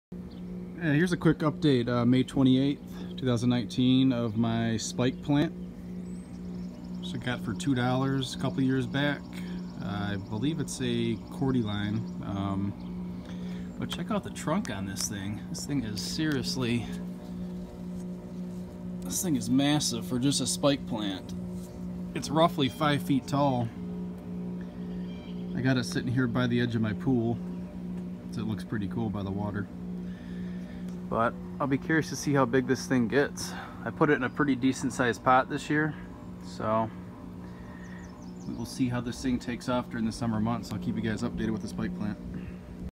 Cordyline Australis (Dracaena Spike/Spike Plant)
My Spike plant outdoors for the summer.